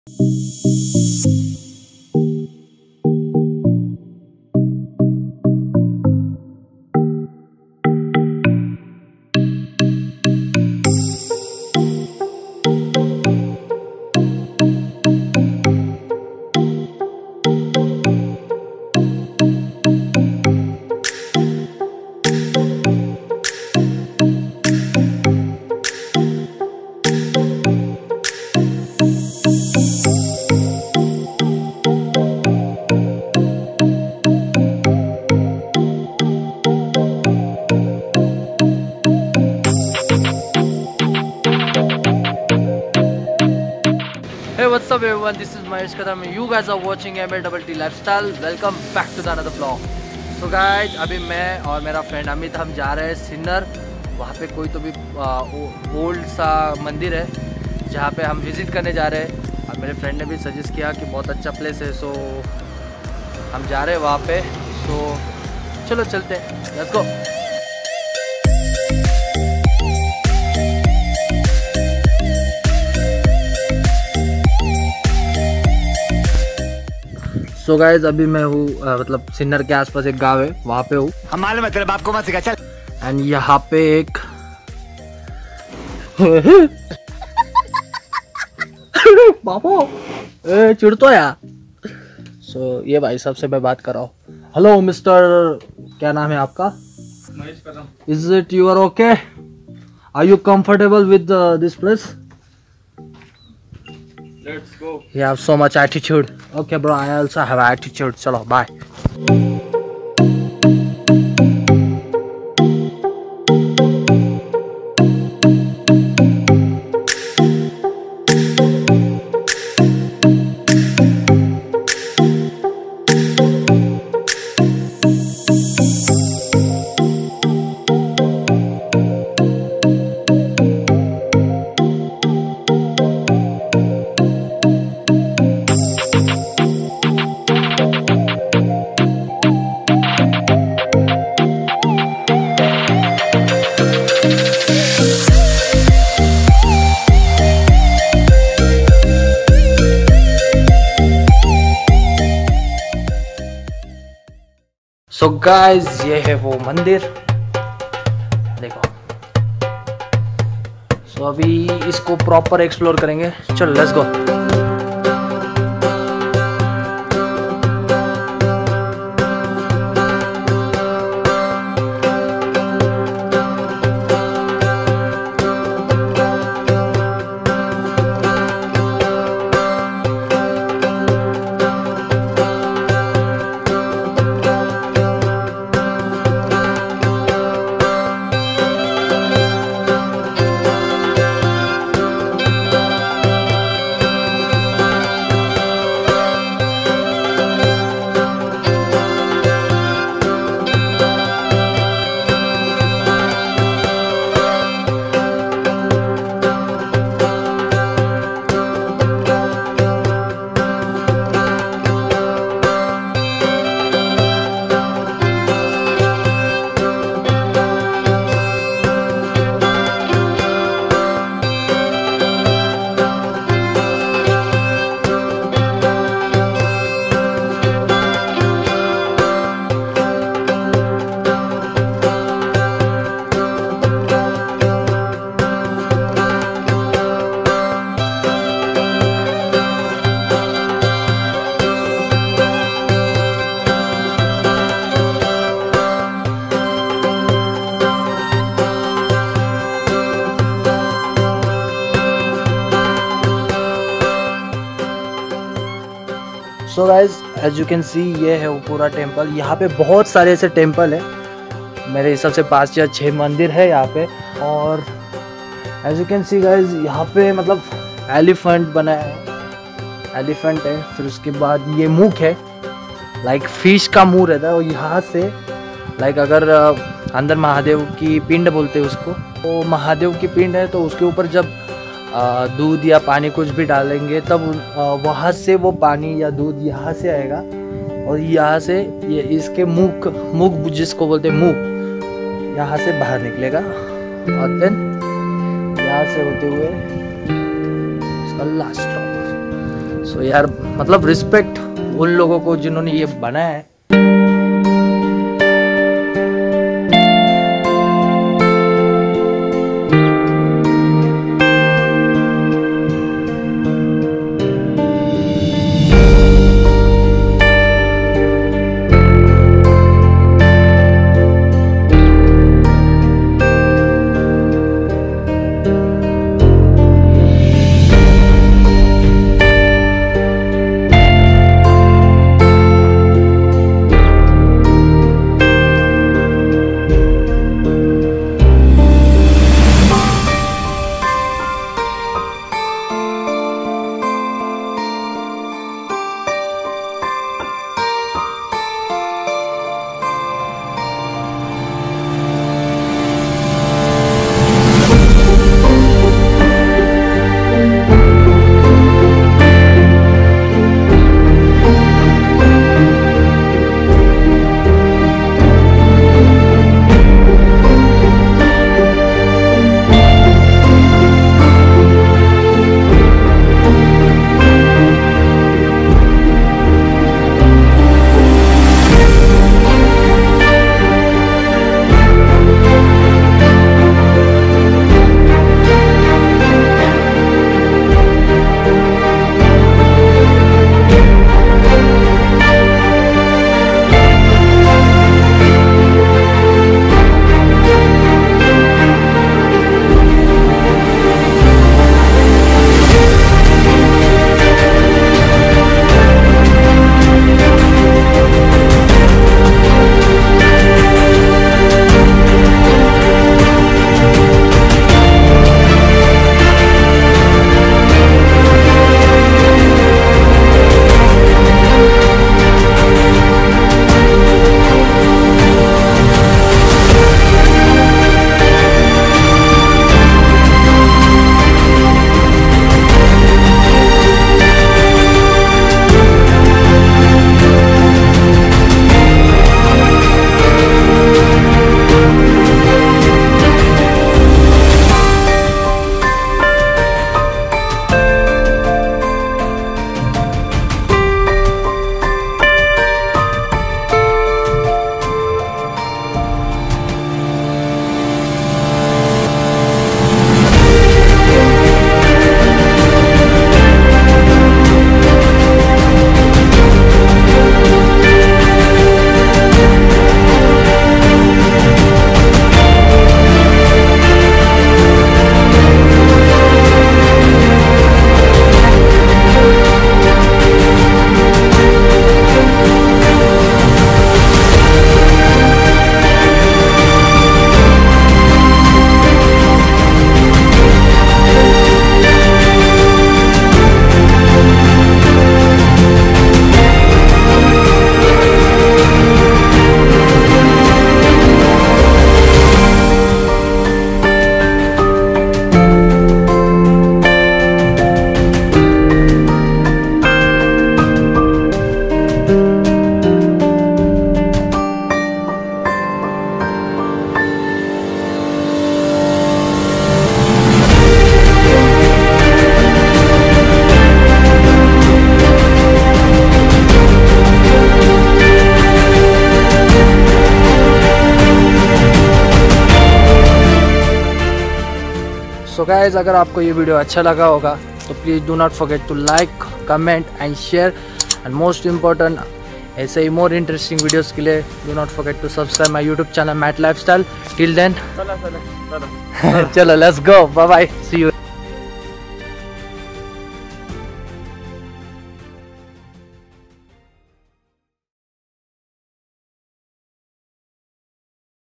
The Undiscovered 12th Century Gift | गोंदेश्वर मंदिर | Matt Lifestyle | Vlog -2
Information: Gondeshwar temple is a very beautiful temple built in the Hemadpanthi style of Architecture. During the Adilshahi rule in Maharashtra.

              This style became very popular and came to be known as the Hemadpanthi style. The Gondeshwar temple is one of the few structures of this style still in good shape. It is a Shaiva Panchayatan, or a group of five temples within a large enclosure.

             The central shrine is dedicated to Lord Shiva and the remaining four around it are temples to Sri Parvati, Sri Ganapati, Surya Bhagwan and Lord Vishnu.
The temple is truly unique in design, perfect in proportion and the stone carvings are exquisitely beautiful. The pillared hall carved with tortoise, the Kurma Avatar of Lord Vishnu, on the ground, is small, elegant and unique in this part of the country. The structure was constructed using the locally available black basalt stone and lime.
              The temple’s origin dates back to 12 – 13th century during Yadava dynasty. Some sources claim that Raj Govinda of Yadava dynasty built this great temple. It lies 30 km southeast of Nashik city on the Nashik – Pune Highway. Gondeshwar Temple is situated about 2 km from Sinnar Bus Station and it represents the best example of the Hemadpanti style of architecture.